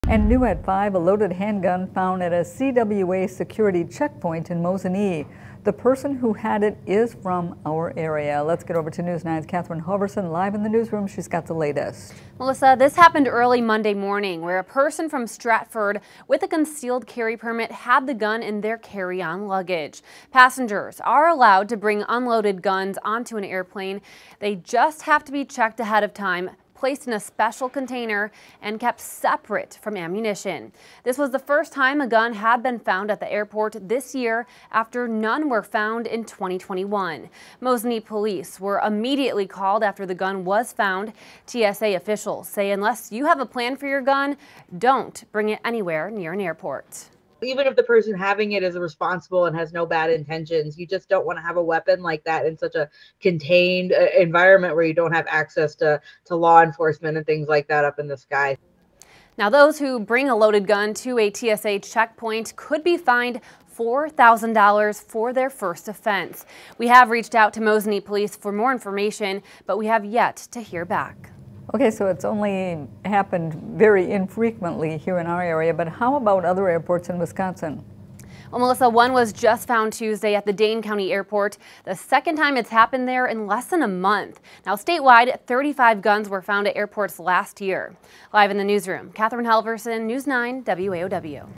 TSA finds handgun in carry-on luggage at Central Wisconsin Airport
The Transportation Security Administration (TSA) says they stopped a gun from being brought on to a plane on Monday at Central Wisconsin Airport.